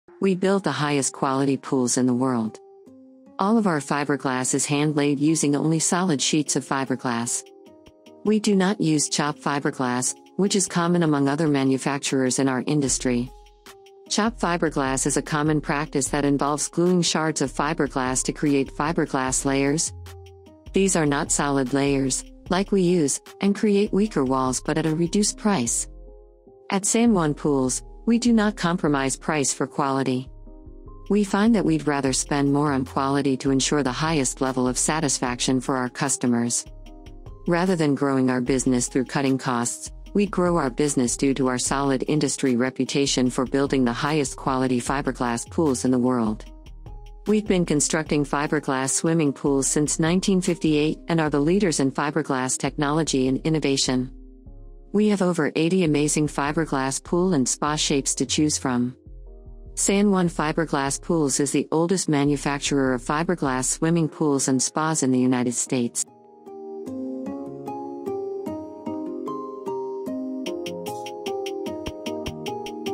We Build The Highest Quality Pools in The World
We Build The Highest Quality Pools in The World. All of our fiberglass is hand laid using only Solid Sheets of Fiberglass. We do not use Chop Fiberglass,  which is common among other manufacturers in our industry.

Chop Fiberglass is a common practice that involves gluing shards of fiberglass to create fiberglass layers. These are not solid layers (like we use) and create weaker walls but at a reduced price.

At San Juan Pools,  we do not compromise price for quality. We find that we'd rather spend more on quality to ensure the highest level of satisfaction for our customers. Rather than growing our business through cutting costs,  we grow our business due to our solid industry reputation for building the highest quality fiberglass pools in the world!

We've been constructing fiberglass swimming pools since 1958 and are the leaders in fiberglass technology and innovation. We have over 80 amazing Fiberglass Pool and Spa shapes to choose from.

San Juan Fiberglass Pools is the oldest manufacturer of fiberglass swimming pools and spas in the United States.

We’d love to give you a free estimate today!